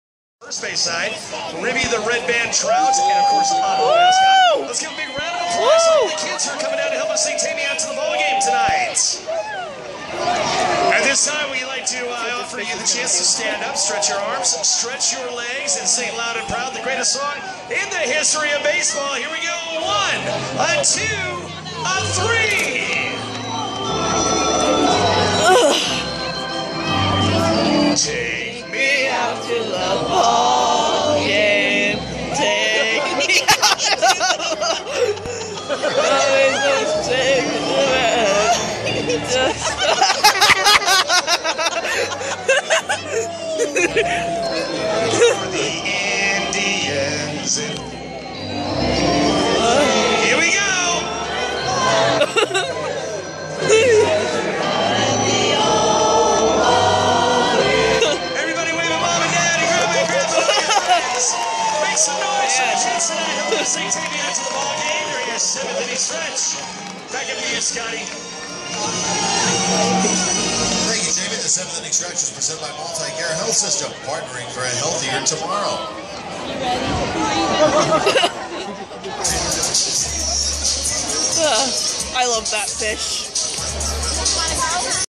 I can't even think of a title for this video.
This was recorded at a Spokane Indian's game. I almost forgot to upload the video completely.